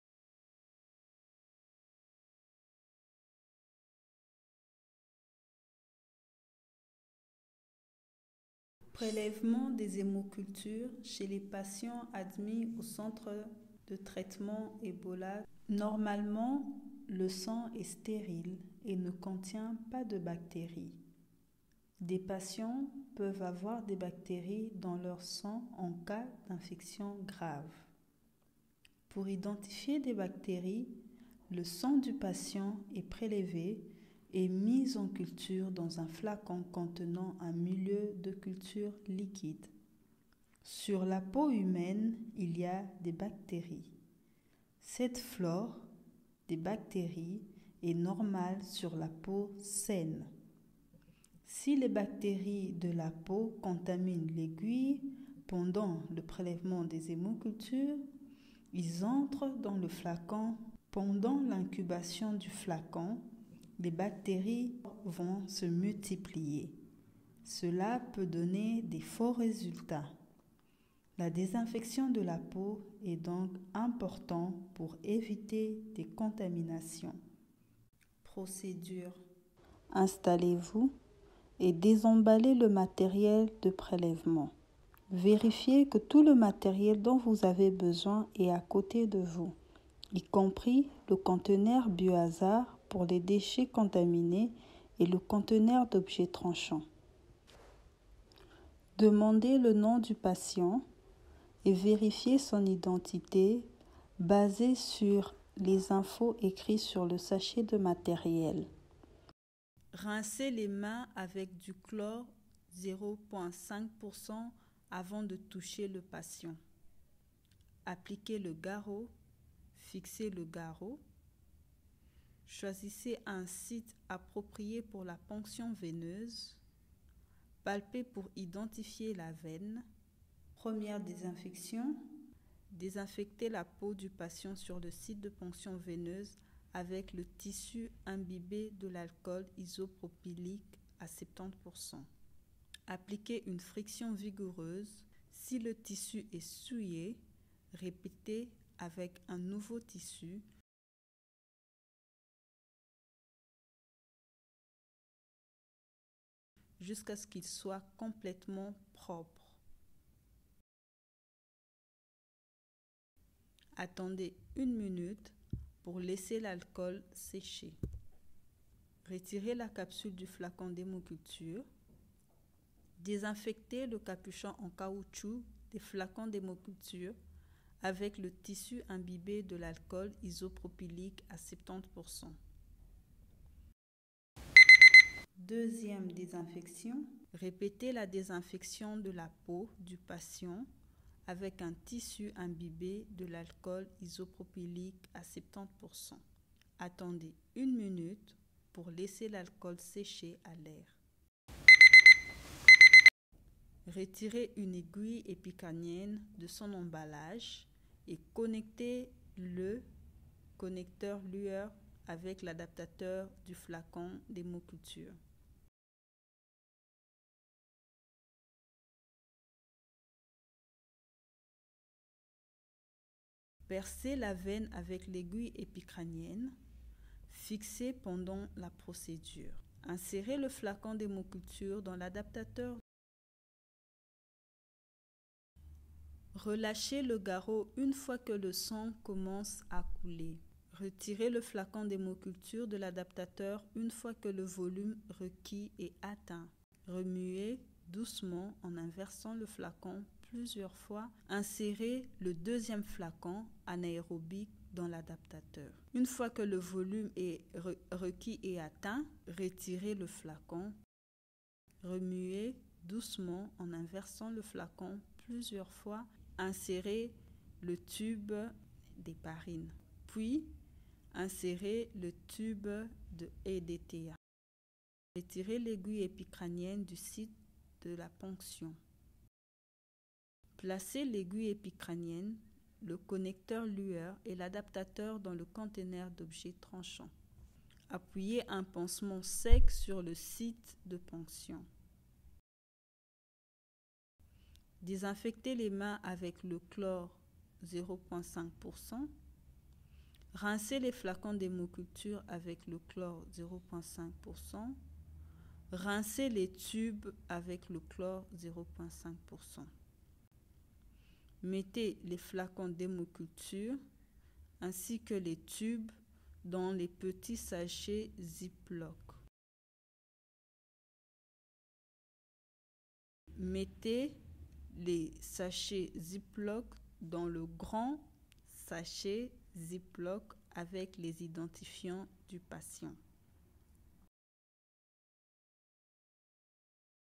Prélèvement des hémocultures chez les patients admis au Centre Traitement Ebola
Cette procédure décrit comment faire le prélèvement des hémocultures chez des patients admis dans un centre de traitement Ebola en utilisant le système BacT/ALERT® (bioMérieux, Marcy-L’Etoile, France).